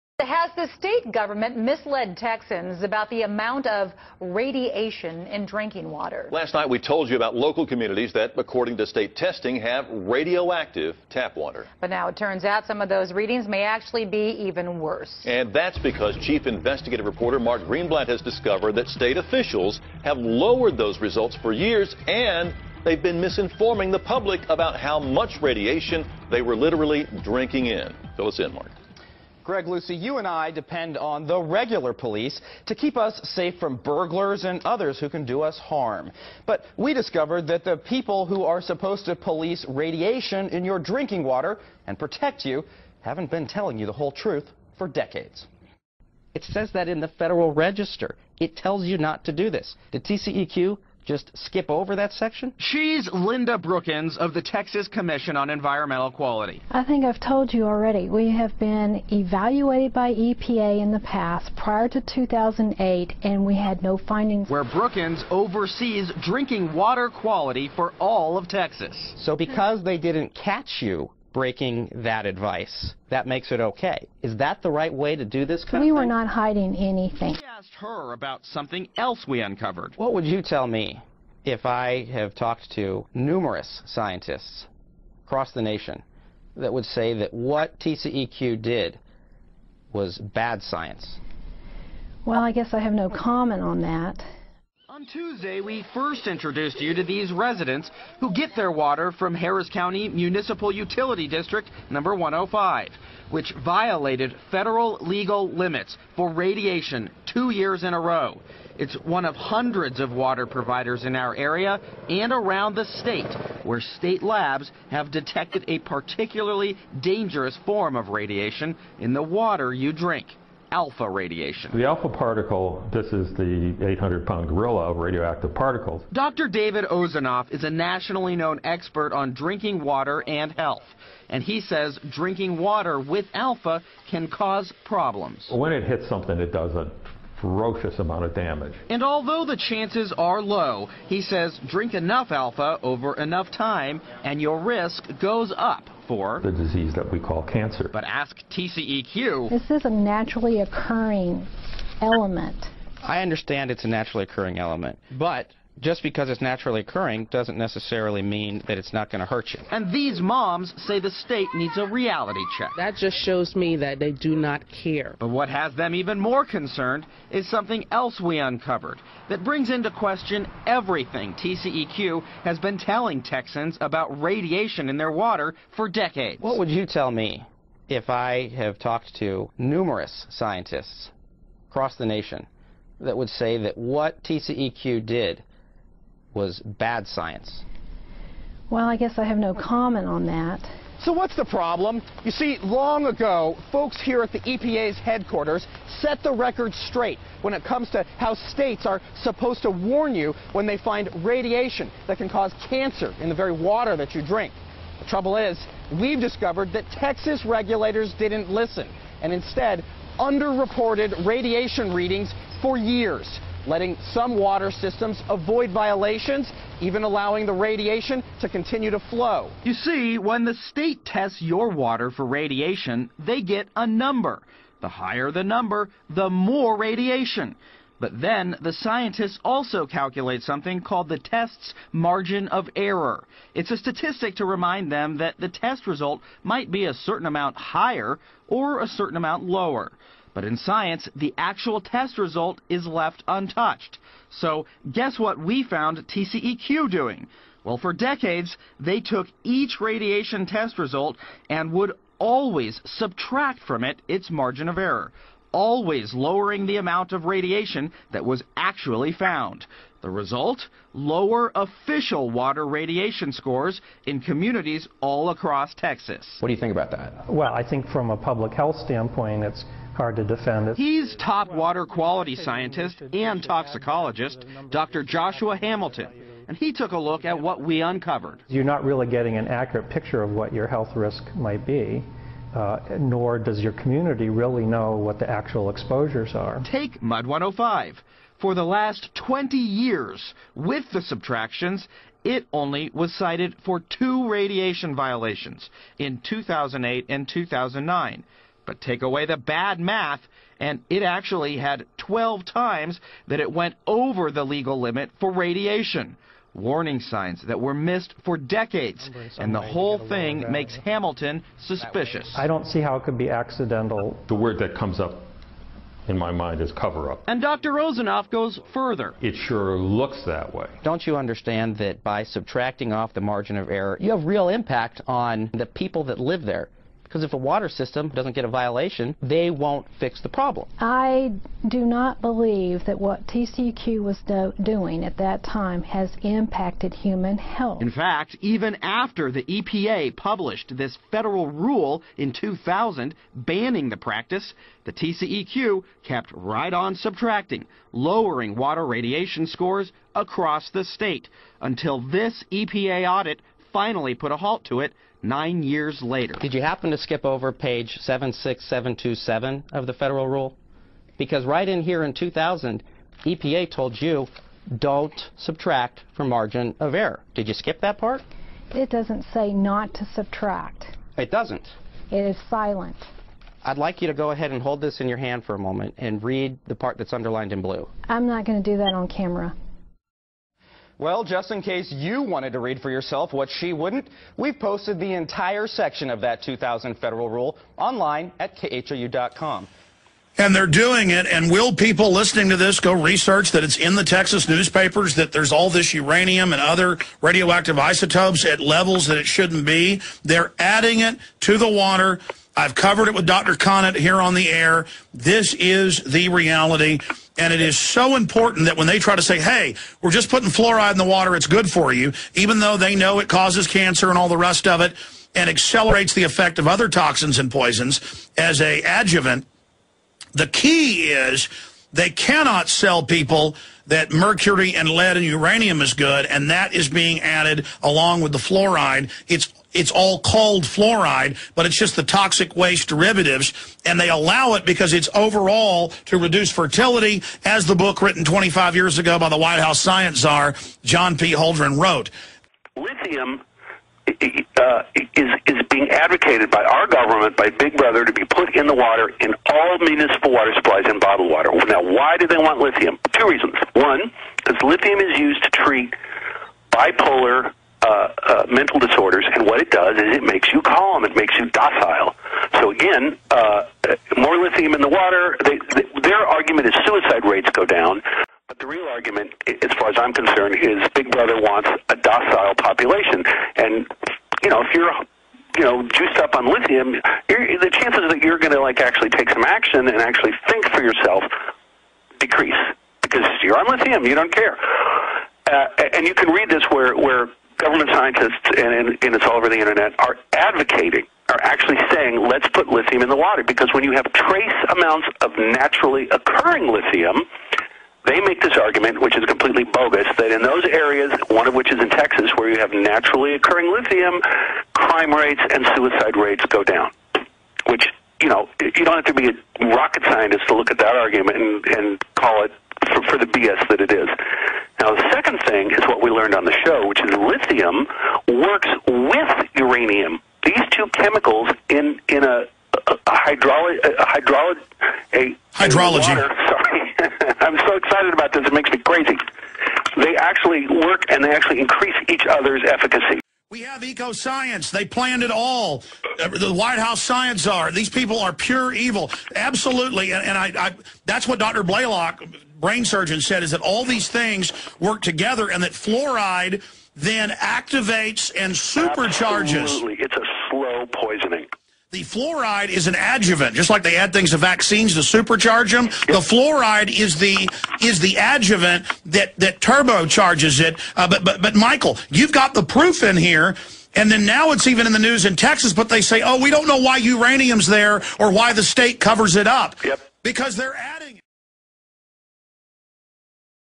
Radioactive Drinking Water!!! 2/2
The State Government of Texas has hidden the fact that radioactive isotopes are in it's citizens'  drinking water.

God Help Us All.